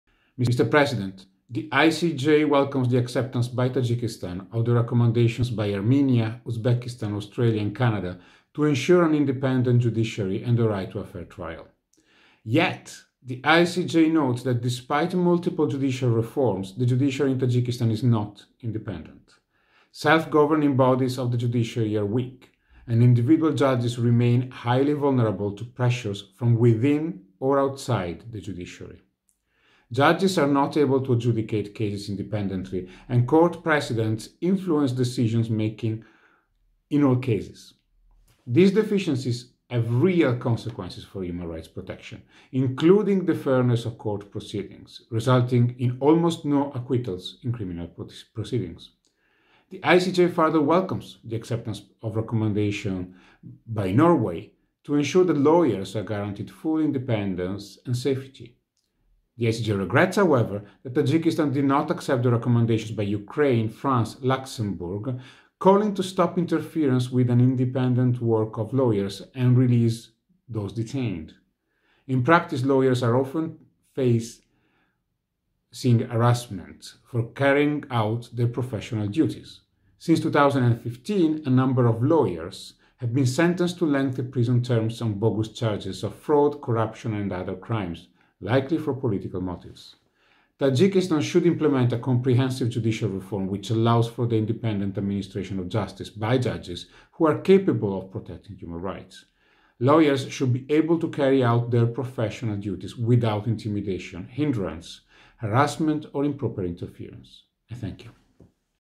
The situation of human rights in Tajikistan
Oral statement of the International Commission of Jurists (ICJ) before the UN Human Rights Council during the examination of the UPR report on Tajikistan